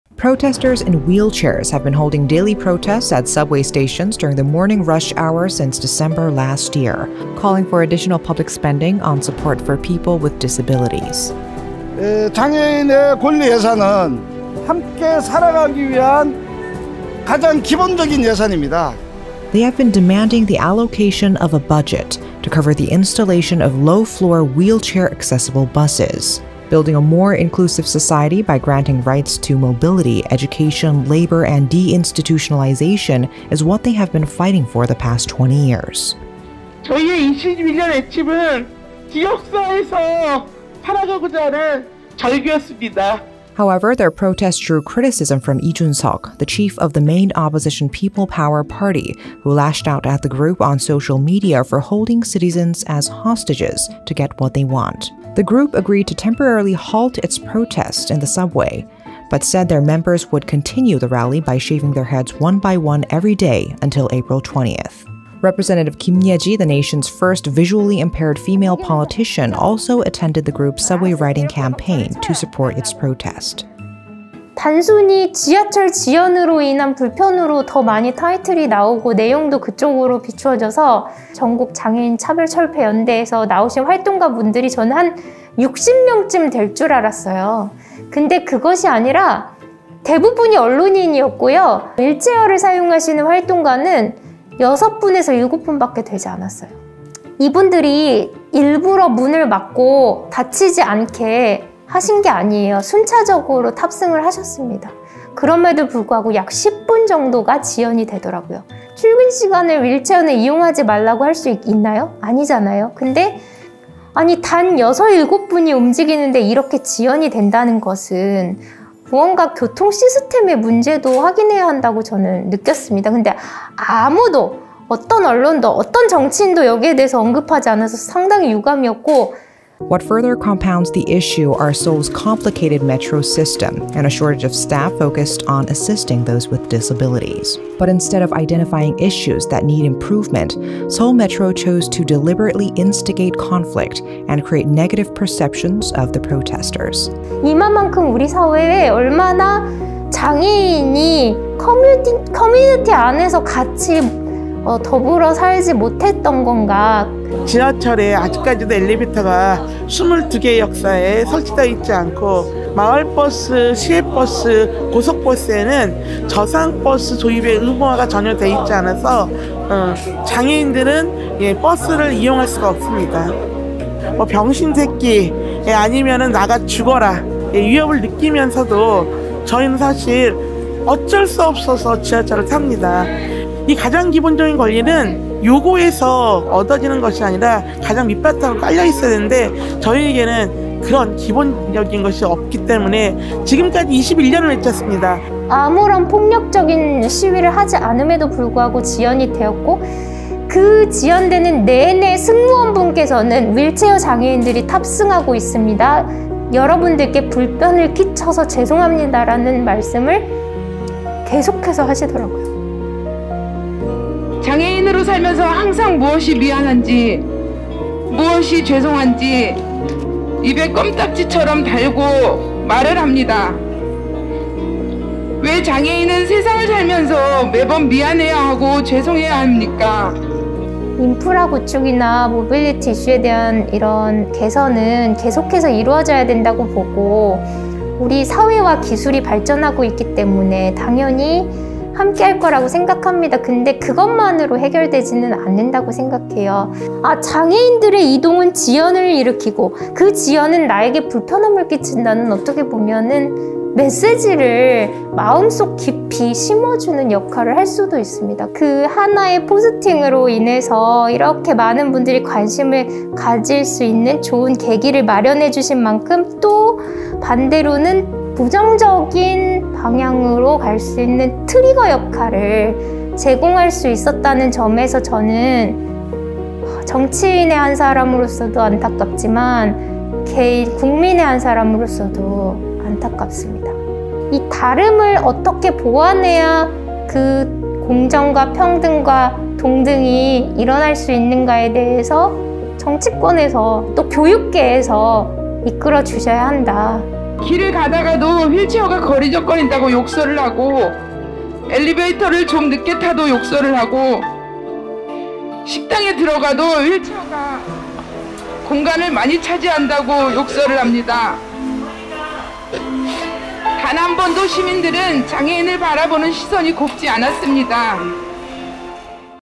People with disability shave head to protest discrimination
Protesters in wheel chairs have been holding daily protests at subway stations during the morning rush hour since December last year,  calling for additional public spending on support for people with disabilities.

They have been demanding the allocation of a budget to cover the installation of low-floor wheelchair accessible buses. Building a more inclusive society by granting rights to mobility, education, labor and deinstitutionalization is what they have been fighting for the past 20 years.

However, their protests drew criticism from Lee Jun-seok,
the chief of the main opposition People Power Party (PPP), who lashed out at the group on social media for holding citizens as"hostages" to get what they want.

The group agreed to temporarily halt its protests in the subway but said their members would continue the rally by shaving their heads one by one every day until April 20.